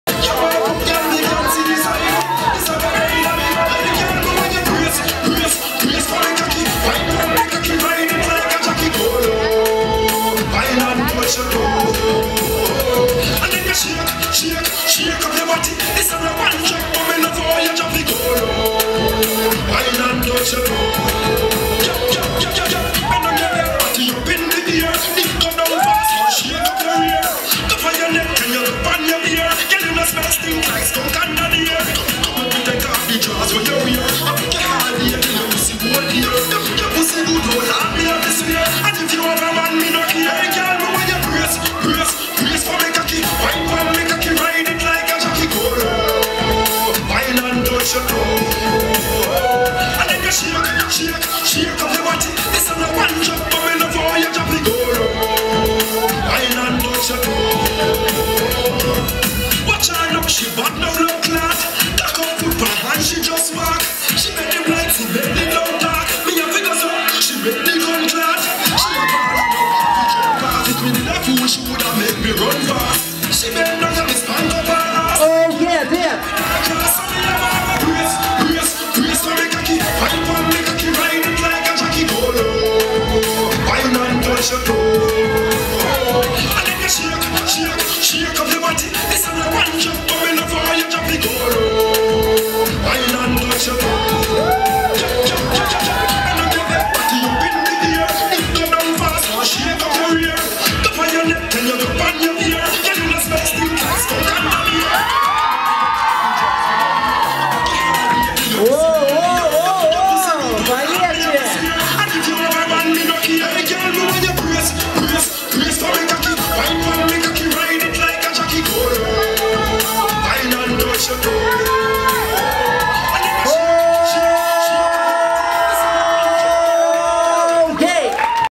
DANCEHALL Round2 - Мару vs Влад
Финальные соревнования танцевального лагеря в Барнауле
FORMAT DANCE CAMP 2015